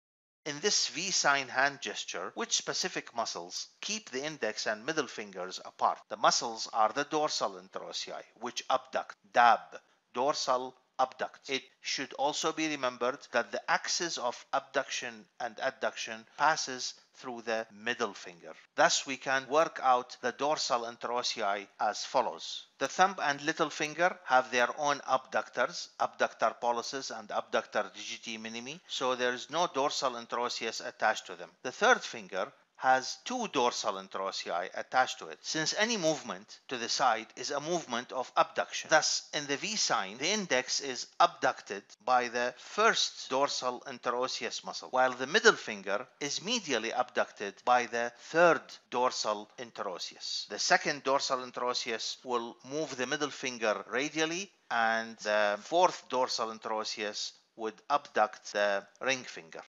Winston Churchills V sign muscles
After watching this video, you will be able to identify the muscle(s) that are used in gesturing Winston Churchill's V sign.

Upper limb